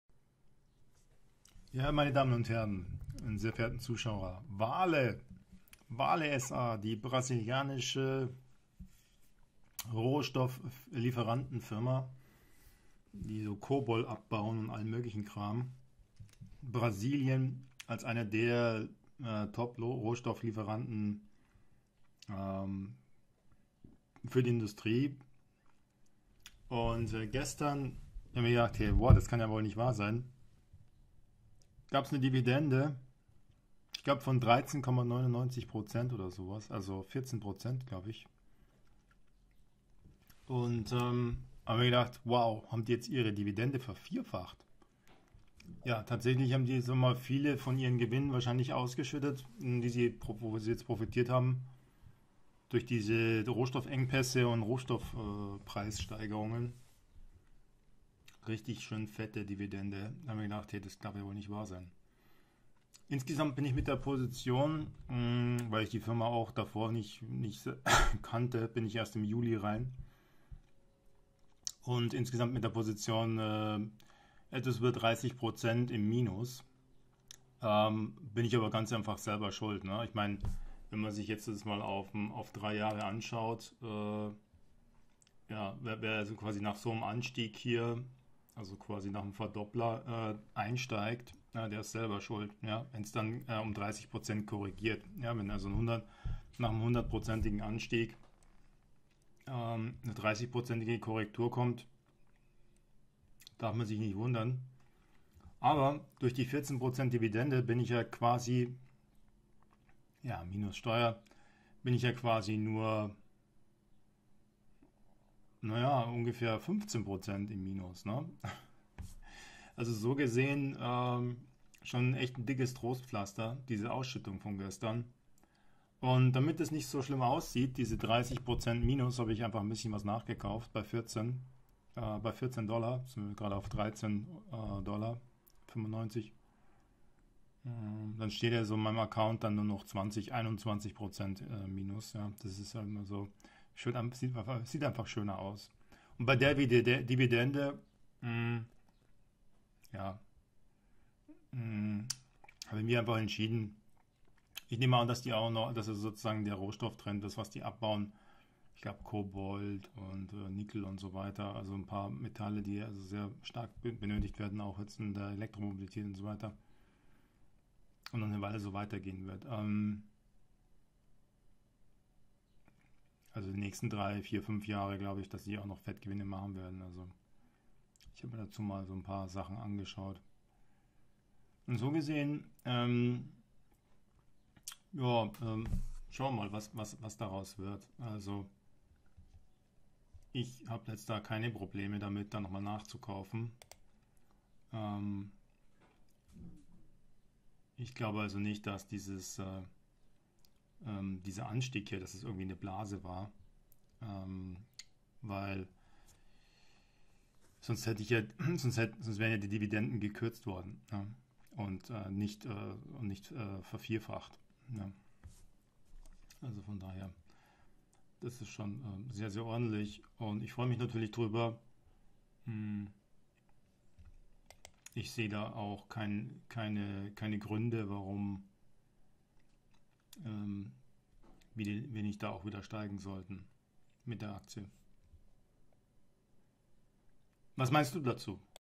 VALE SA Aktie hat knapp 14% Dividende ausgeschüttet - Welche Dividendenaktien soll ich kaufen?
WAU, ich war gestern wirklich überrascht als ich festellte, dass meine -30% Position bei VALE SA mit einer 14% Dividende kompensiert wurde.  💥 Dein Aktiendepot Vergleich 2020 - Wertpapierdepot-Empfehlungen - Depot kostenlos eröffnen❗